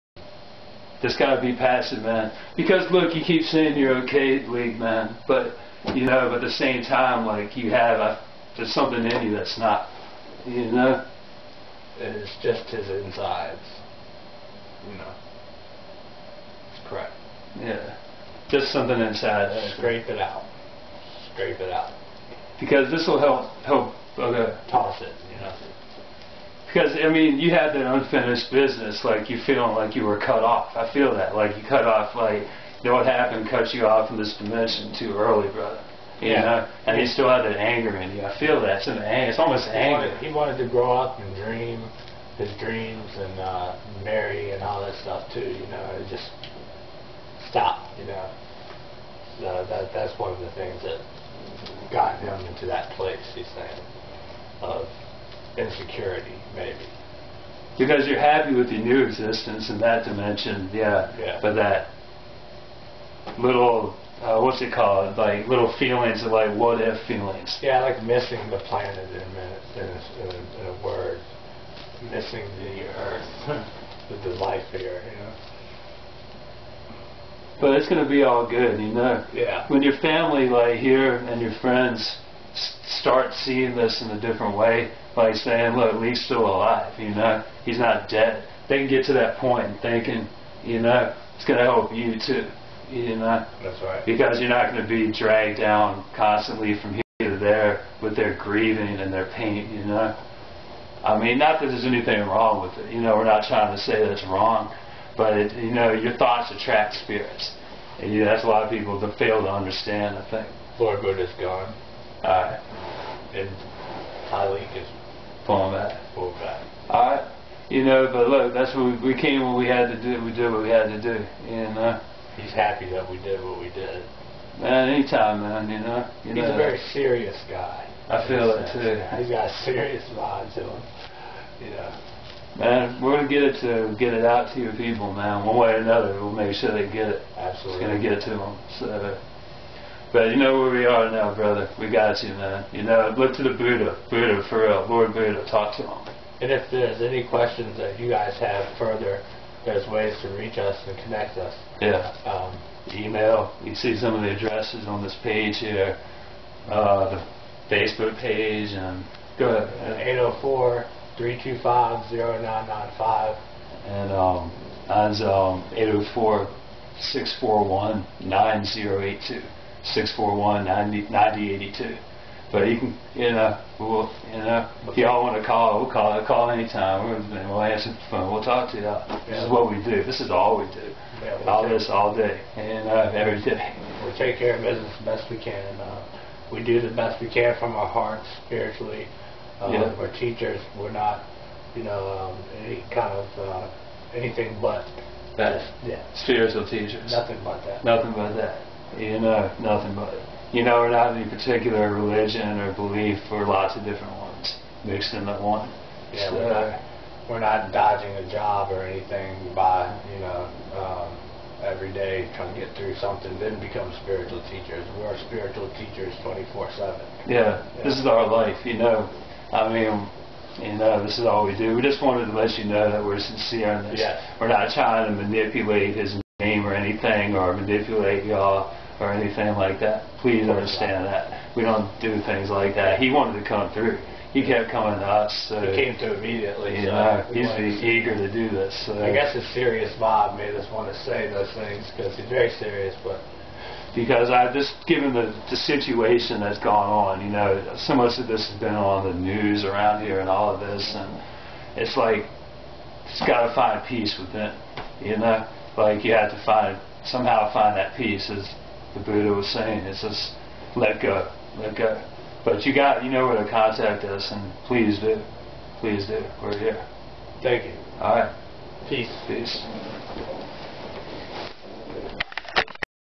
Tahliek - (part 2 of 2)
Christy was talking about Powhatan County and living in the country, and racism, when this dude Tahliek came through.  He had come through to me last year, but at that time we hadn't thought of the idea of filming videos & putting them up on the Net, so we didn't know how to reach his people.  What happened with him was big news here in Richmond a couple of years ago.  A couple of white dudes shot him and all these accusations of racism were going back and forth.  Liek wanted to contact his family and friends and let them know he is alright in his new existence.  He is also kind of confused himself and a little upset at why this went down - as I called it - "beef gone overboard".  Also something that he mentioned to me the night before we filmed this video:  he is also very concerned about his friend Courtney who was also shot that night.  Liek was showing me Courtney's emotional inner pain and his anger.  To Courtney if you watch this:  Liek is with you.  He has been watching over you.  He feels your anger and abandonment and shame and rage.  He loves you, my brother.  Even in death.  If you ever want to talk with us, our #'s are in the video.  Either way, Jeremy & I are here to help.  To Tahliek's family & friends:  Jeremy and I hope this helps you.  Tahliek has also been with you the whole time.  If you have any questions, please call Jeremy & I, please don't feel funny about calling us.  Please forgive us for putting this out there like this...but Tahliek desperately wanted to reach you all, and this was the best way we could do it.   Know that we made every effort to interpret what Liek was showing us correctly; often spirits use images and impressed thought to tell the story; we hope that things will be as accurate as humanly possible.  May peace be upon you.